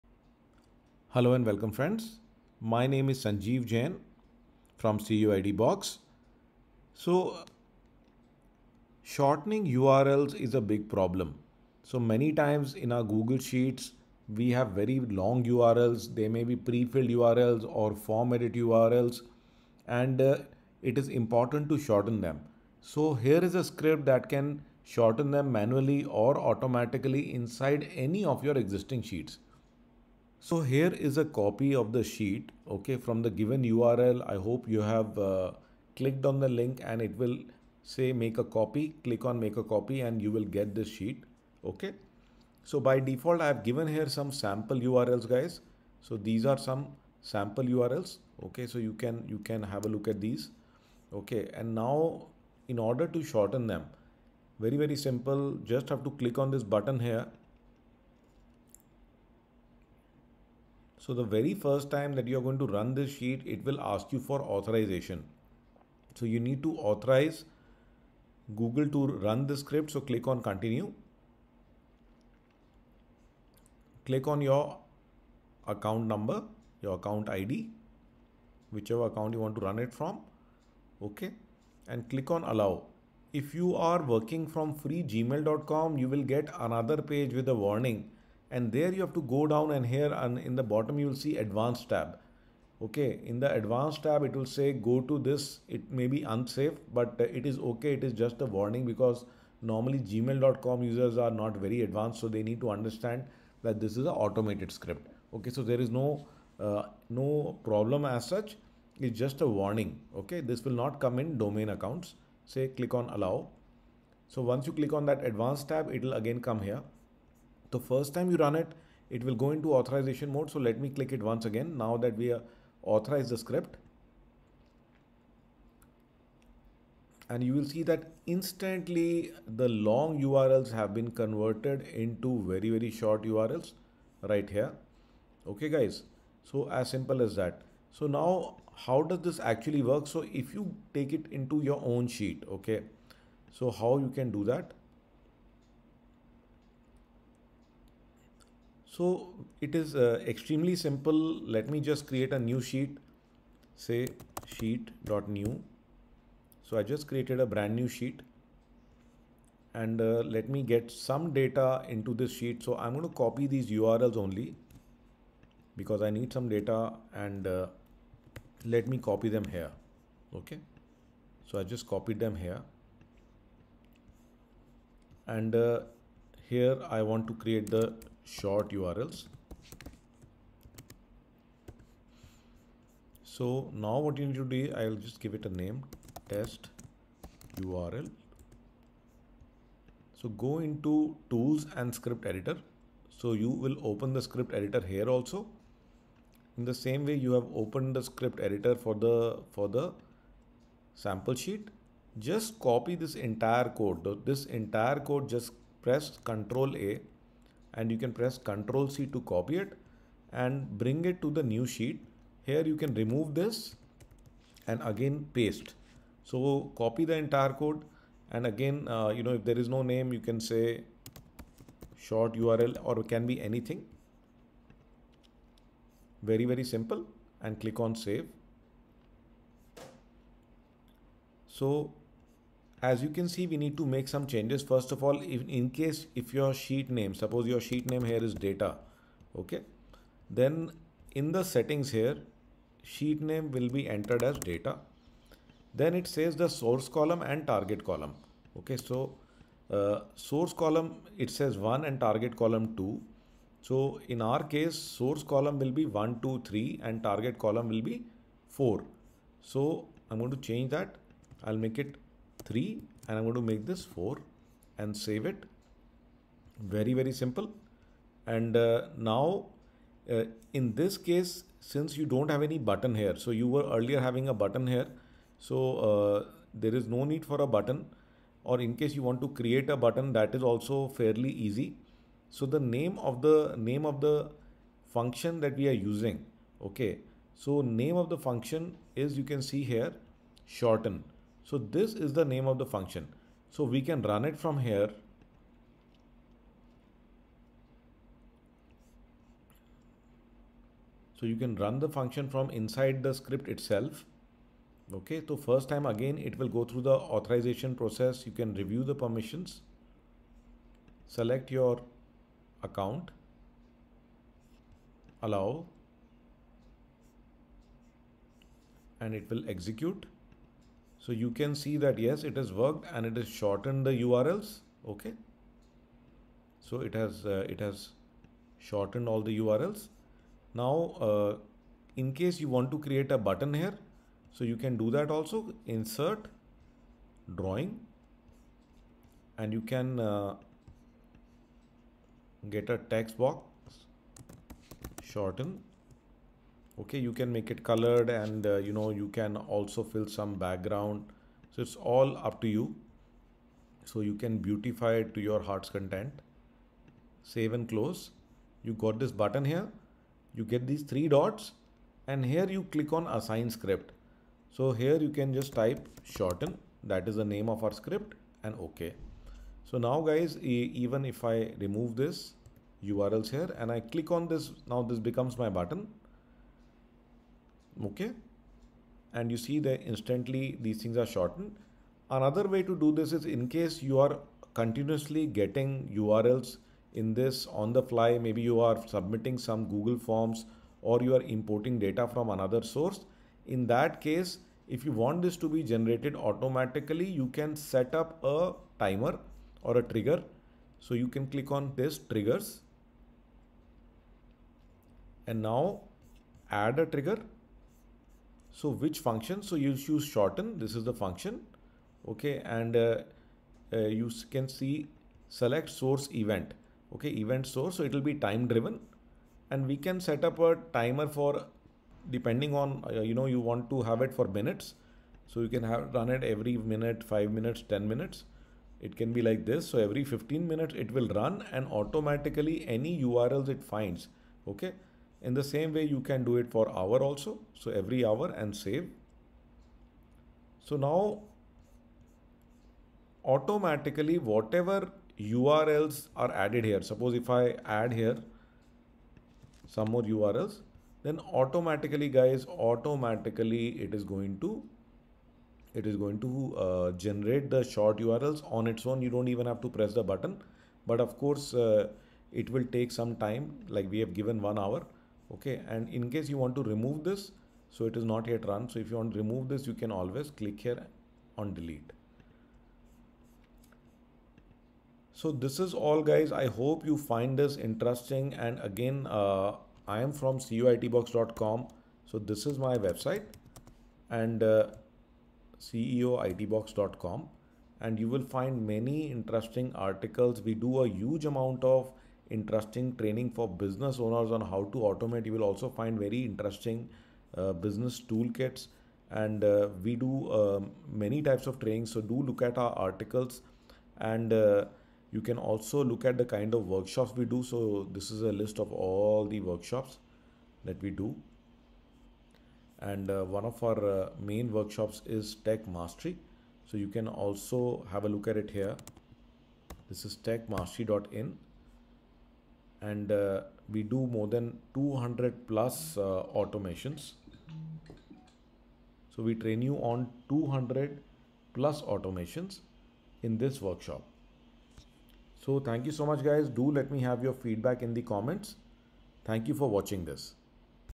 Shorten URLs Automatically in Google Sheets
Shortening multiple URLs can consume a lot of time and effort.
This script will automatically shorten any long URL in any google sheet and shorten the same using tinyurl.com  You can create a button or even setup a trigger to run in automatically. You can add this to any new\existing Google Sheet.